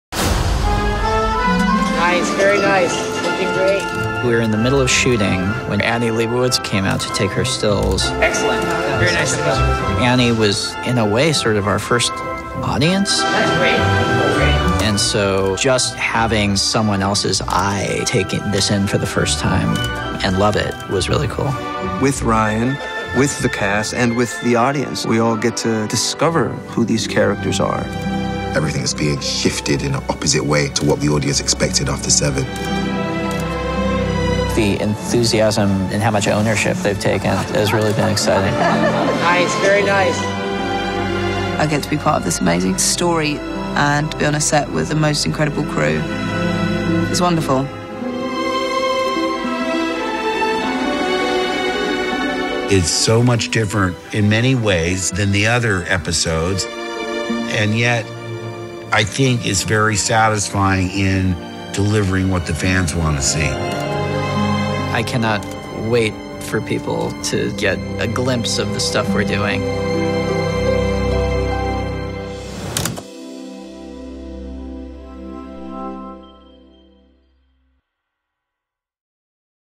Star Wars: The Last Jedi - On Set Exclusive | Vanity Fair
See the cast of "Star Wars: The Last Jedi" reunite for photographer Annie Leibovitz on their Vanity Fair cover shoot.